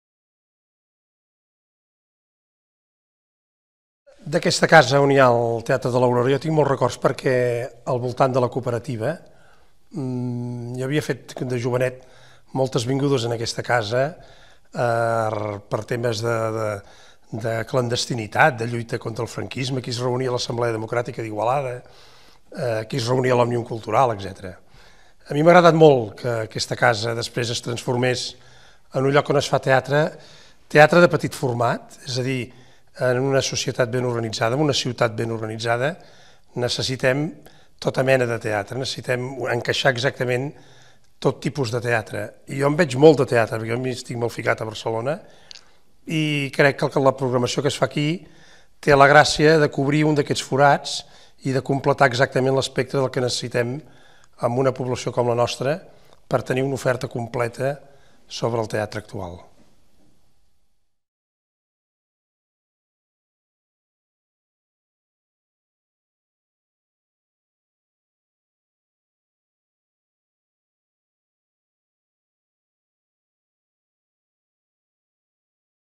Antoni Dalmau i el Teatre de l'Aurora
L'escriptor igualadí Antoni Dalmau parla sobre el Teatre de l'Aurora (Igualada).